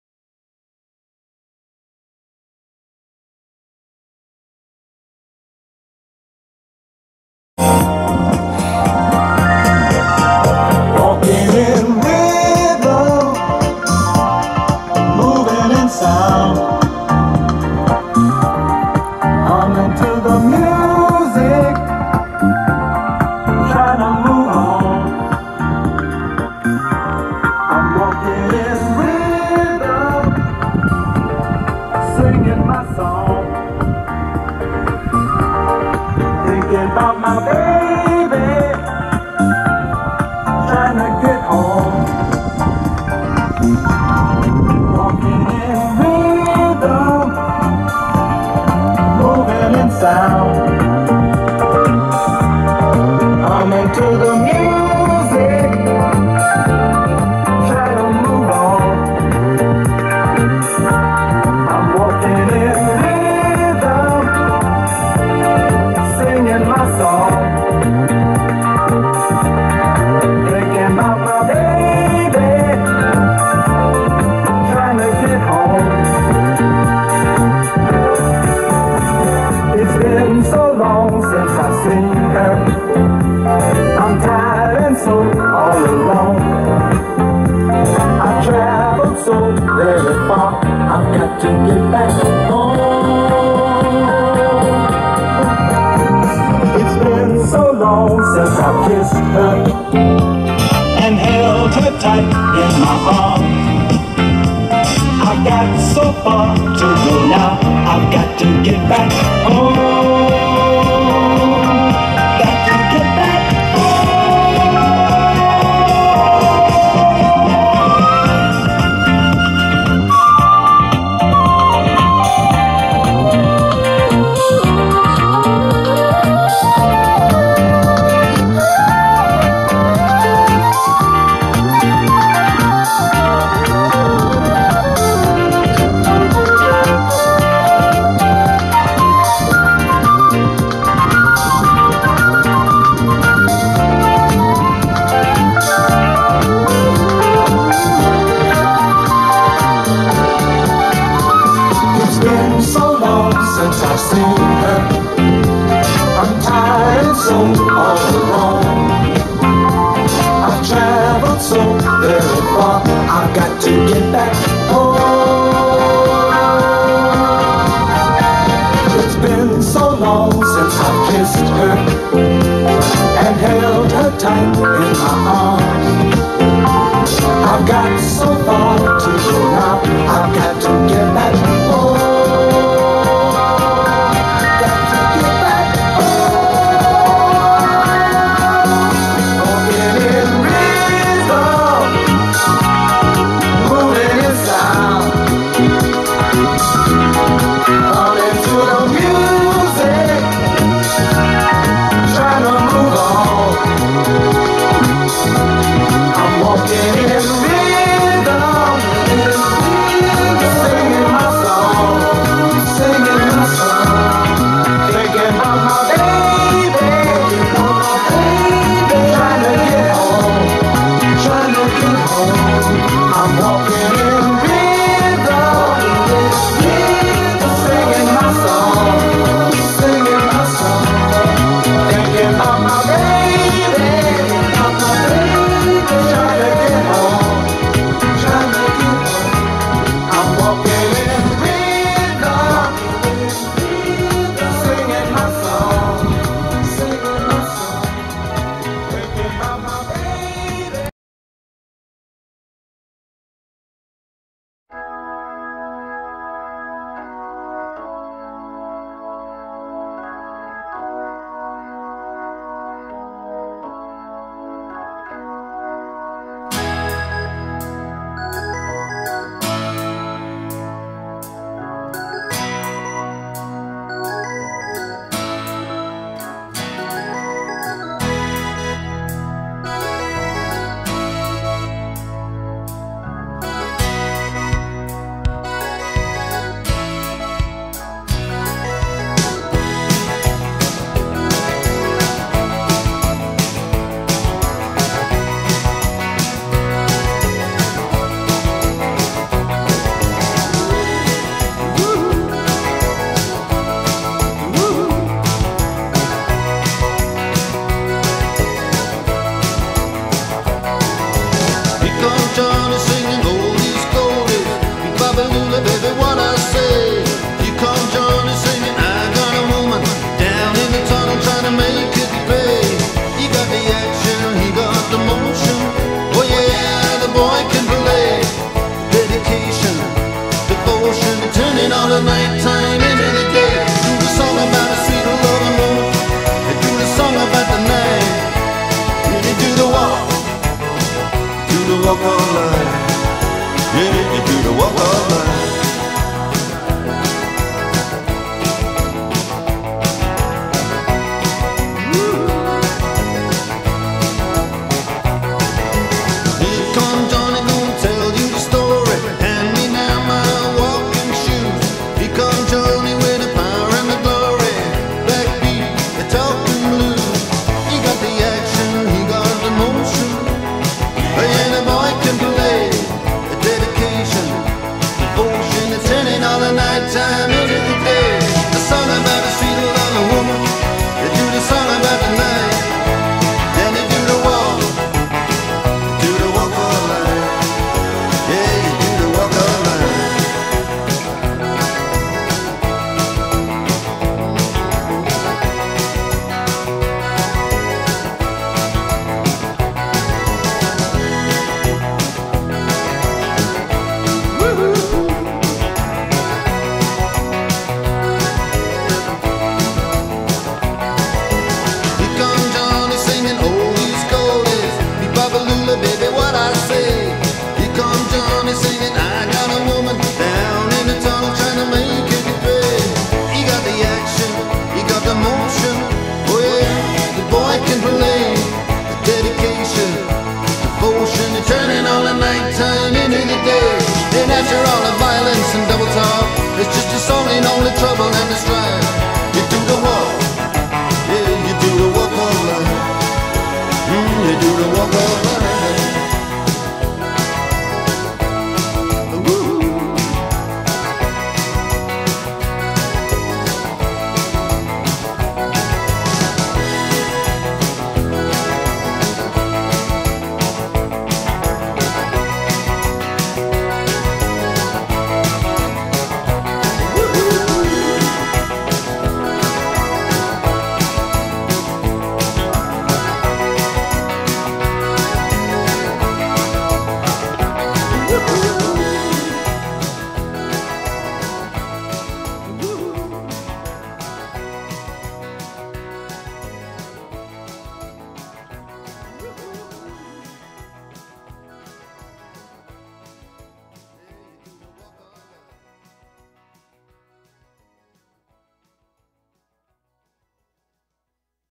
Martins Walk Of Life
Charity Walk remembering Martin - In aid of Cancer UK.